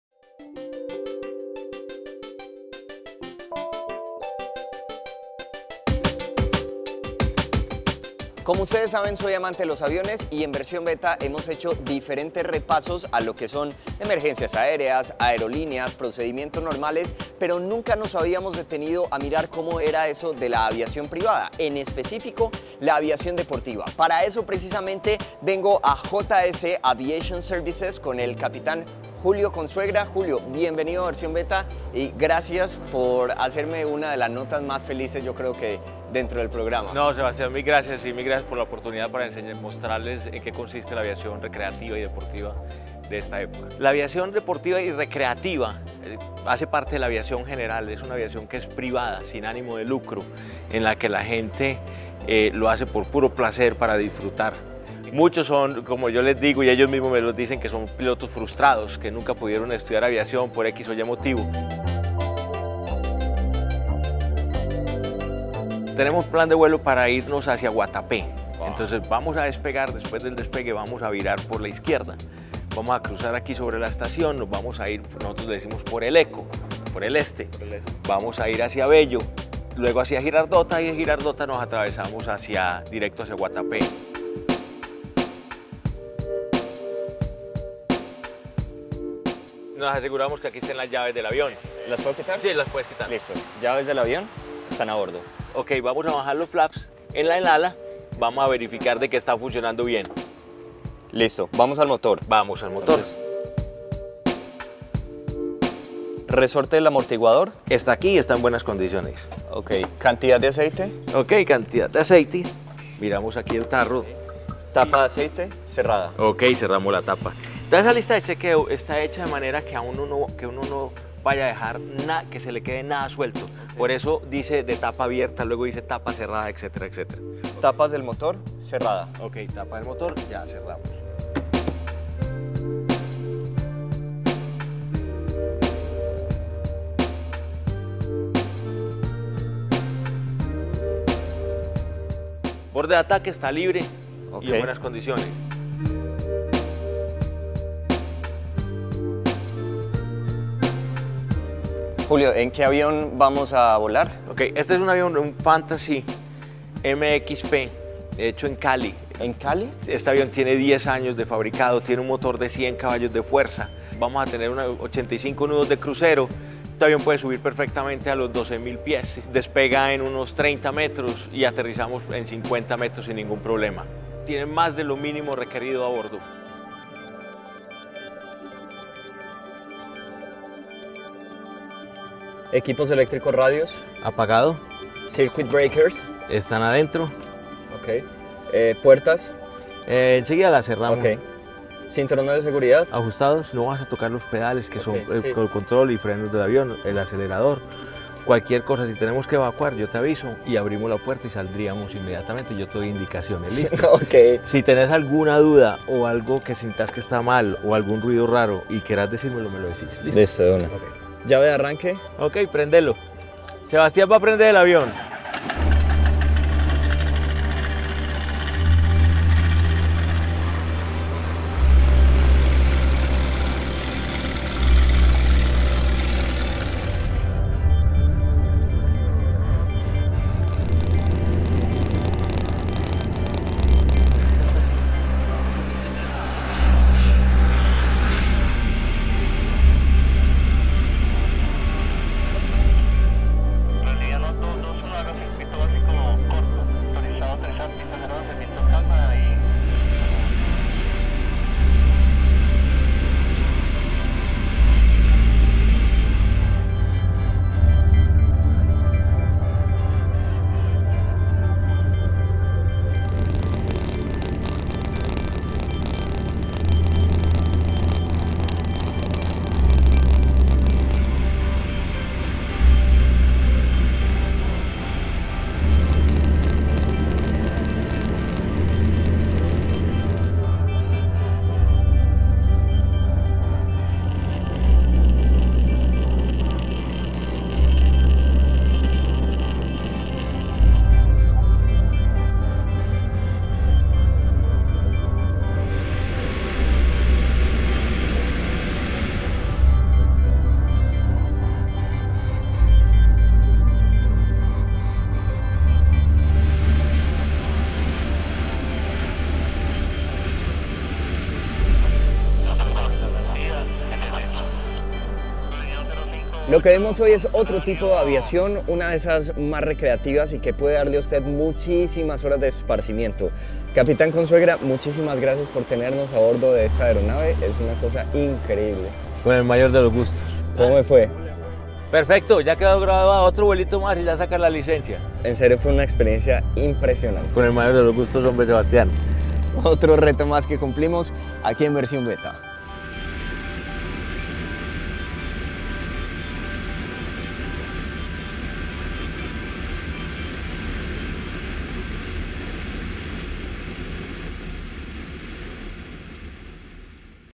Piloto por un día
Ser piloto es más asequible de lo que usted piensa. Sebastián Misas voló junto al capitán Julio Consuegra en un ultraliviano como parte del programa Piloto por un Día.